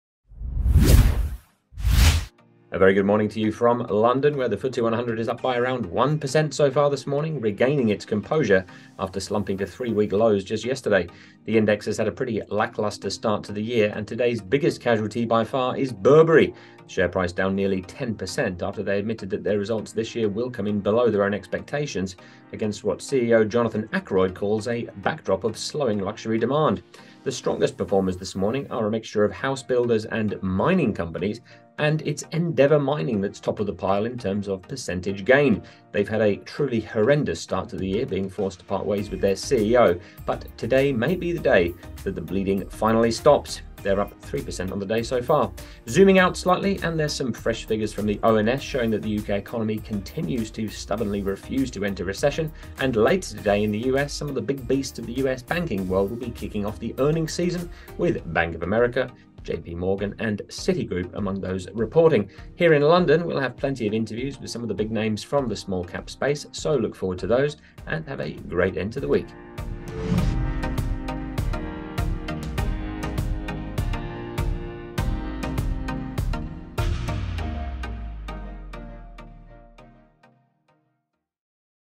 FTSE 100 up sharply after tough start to the year - Market Report
Good morning from London, where the FTSE 100 is up by around about 1% this morning, regaining its composure after slumping to three-week lows just yesterday.

The index has had a pretty lacklustre start to the year and today’s biggest casualty by far is Burberry, the share price is down nearly 10% after they admitted their results this year will come in below their own expectations against what CEO Jonathan Akeroyd calls a “backdrop of slowing luxury demand.”

The strongest performers this morning are a mixture of housebuilders and mining companies and its Endeavour Mining that’s top of the pile in terms of percentage gain. They’ve had a truly horrendous start to the year being forced to part ways with their CEO but today may be the day that the bleeding finally stops – they’re up around 3% on the day.

Zooming out slightly and some fresh figures from the ONS show that the UK economy continues to stubbornly refuse to enter recession and later today in the US, some of the big beasts of the US banking world will be kicking off the earnings season with Bank of America, JPMorgan and Citigroup among those reporting.

Here in London we’ll have plenty of interviews with some big names from the small cap space so look forward to those, and have a great end to the week.